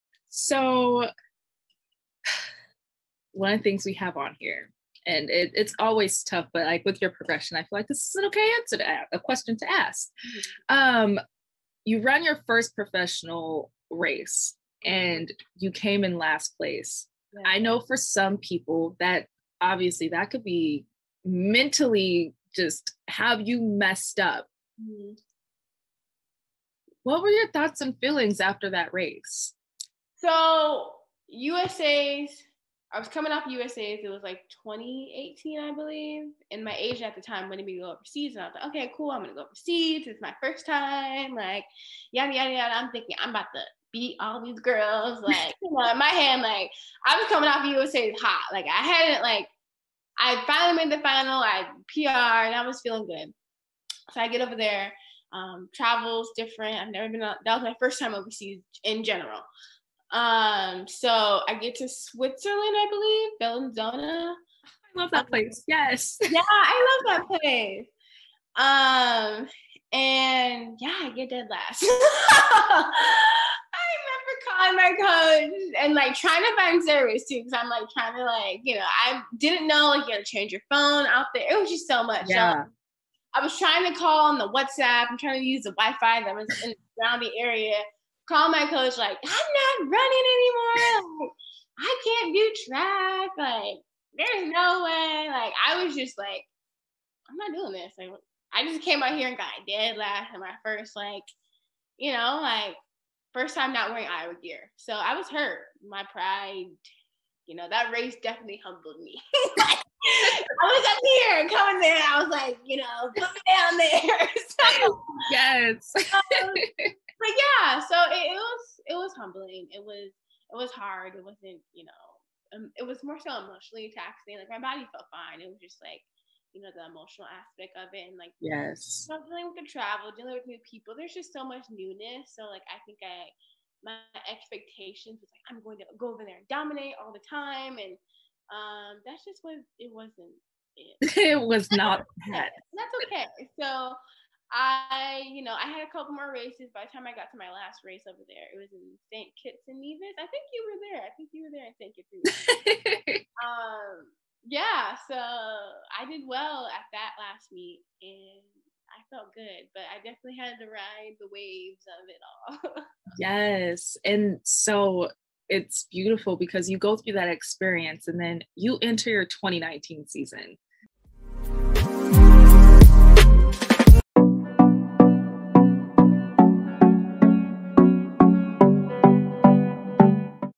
Olympic Motivation going from Last to becoming a CHAMPION! | Crazy Faith Podcast
Powered by Agogie

Brittany Brown defies the odds and goes from coming in last place in her 1st professional race to winning silver at the 2019 Doha World Championships